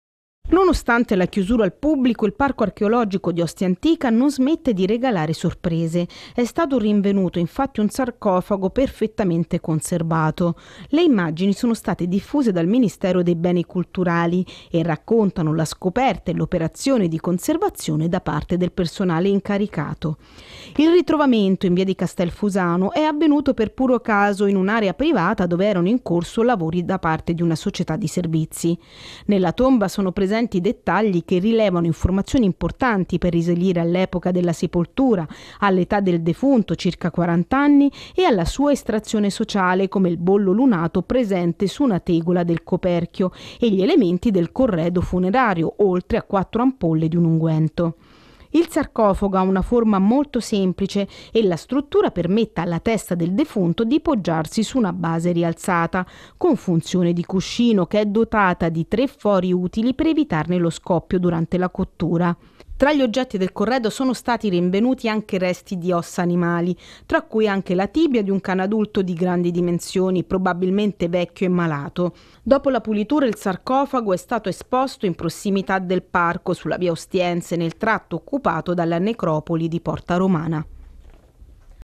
Rinvenuto sarcofago nel Parco Archeologico di Ostia Antica - Canale 10
Il Parco Archeologico di Ostia Antica non smette di regalare sorprese. In via di Castelfusano è stato rinvenuto un sarcofago perfettamente conservato.